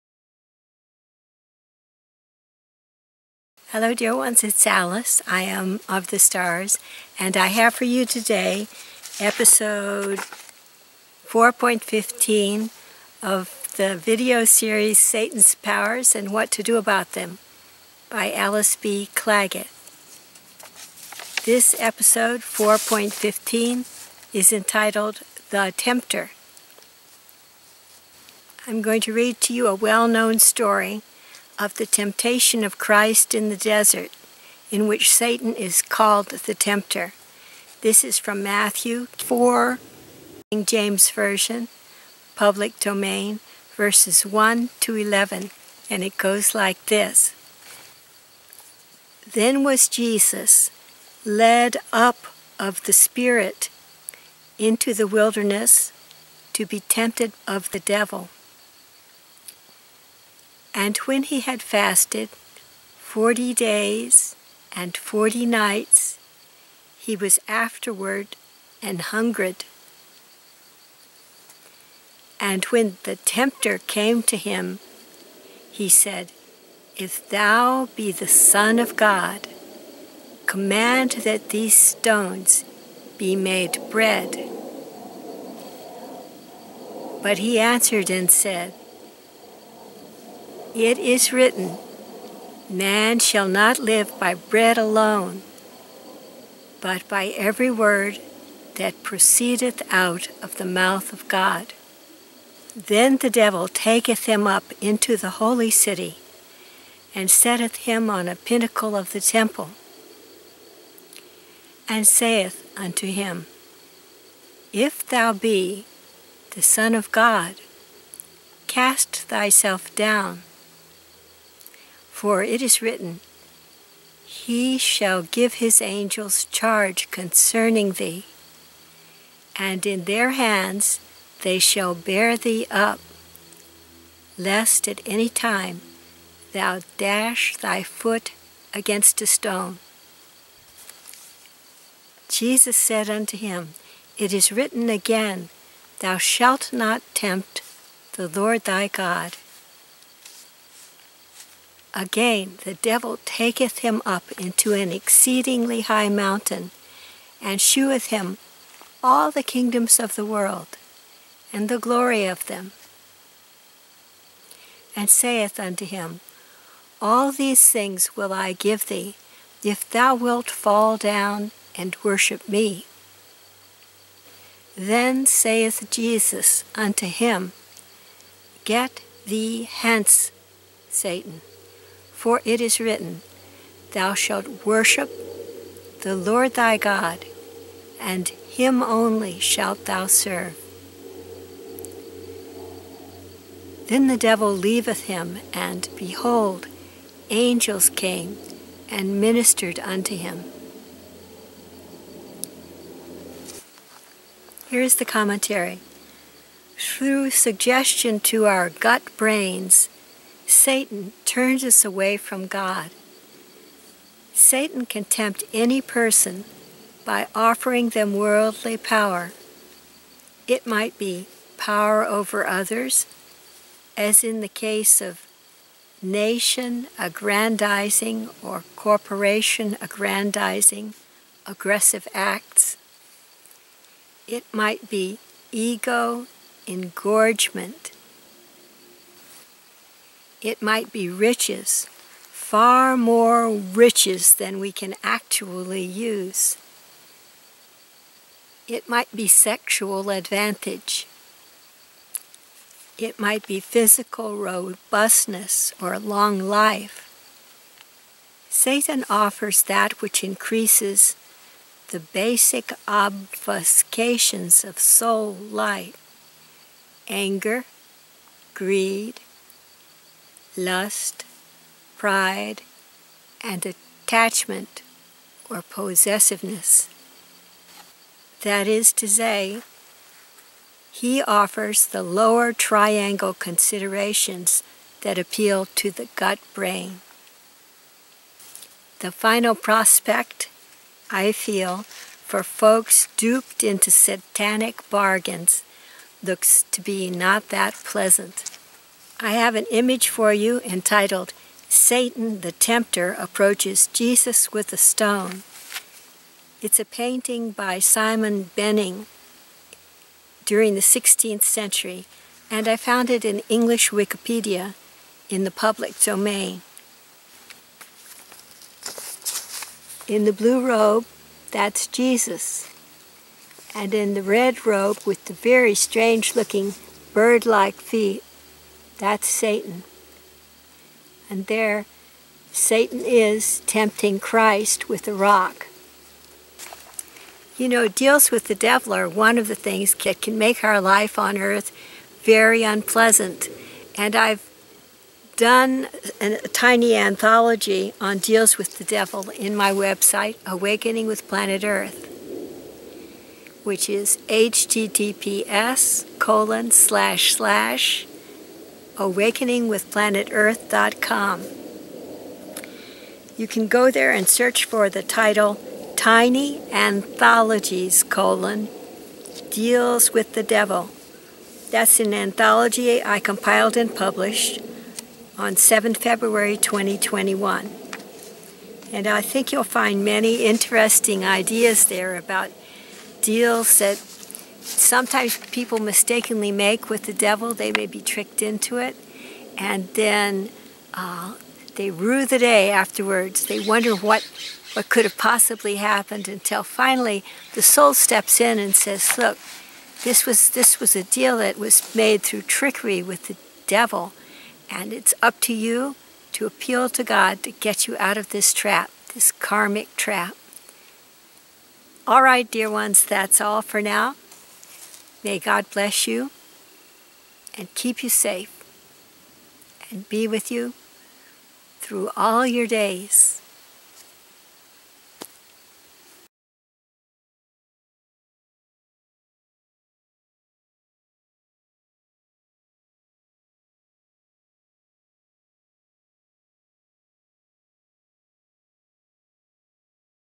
Satan's Powers: 4.15 The Tempter . by Alice B. Clagett
Full Title: "Satan's Powers and What to Do about Them: 4.15 The Tempter" . by Alice B. Clagett
Blog written and published on 10 October 2013; updated with images, text and videos in October and November 2021; video filmed on 22 November 2021 and published on 23 November 2021